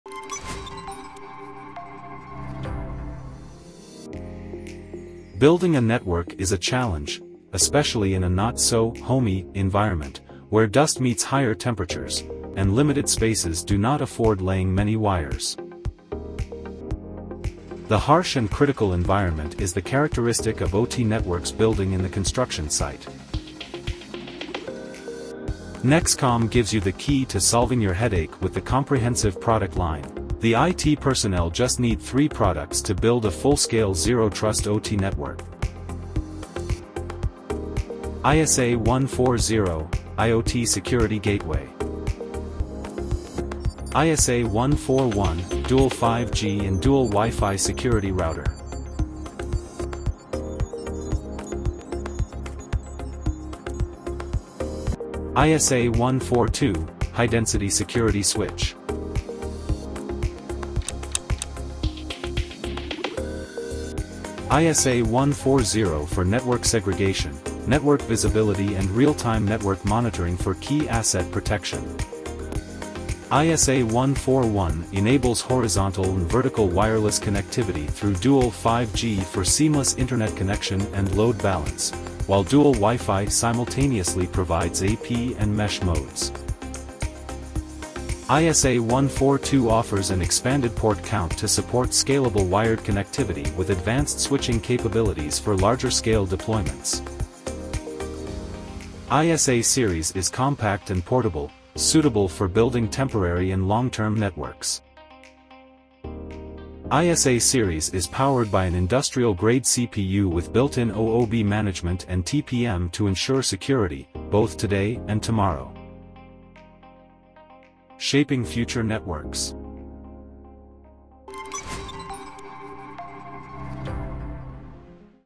Full-Scale OT Network Security with NEXCOM's ISA Series
Powered by Intel Atom x6000 Series industrial-grade processors, ISA Series ensures there are no security breaches in your operations with three mission-oriented appliances:

0:00~0:24 Opening
0:25~1:03 Product Features
1:04~1:37 Application Scenario
1:38~2:05 End